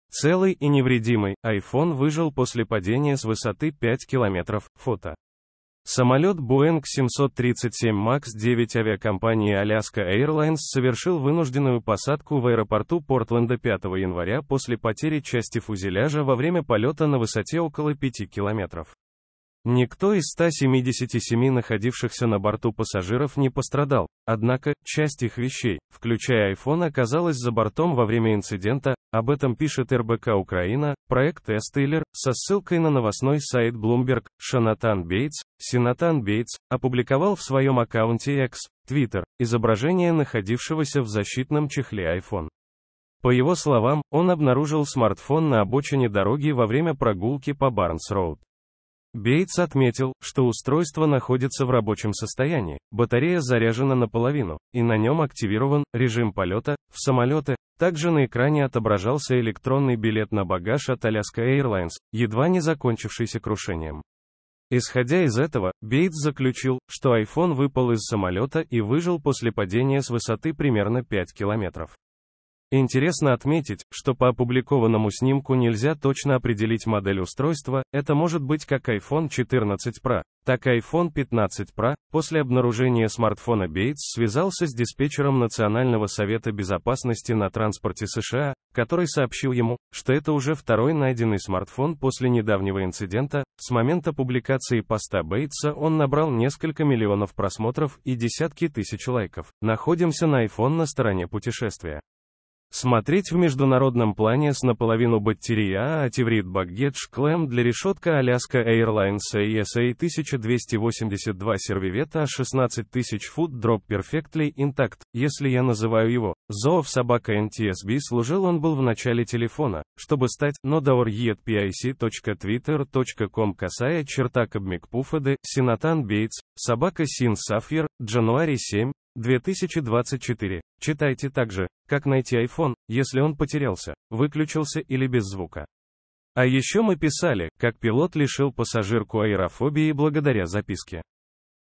Целый и невредимый: iPhone выжил после падения с высоты 5 километров (фото).
iPhone Boeing Самолет Boeing 737 Max 9 авиакомпании Alaska Airlines совершил вынужденную посадку в аэропорту Портленда 5 января после потери части фюзеляжа во время полета на высоте около 5 километров. Никто из 177 находившихся на борту пассажиров не пострадал, однако, часть их вещей, включая iPhone оказалась за бортом во время инцидента. Об этом пишет РБК-Украина (проект Styler) со ссылкой на новостной сайт Bloomberg. Шонатан Бейтс (Seanathan Bates) опубликовал в своем аккаунте X (Twitter) изображение находивш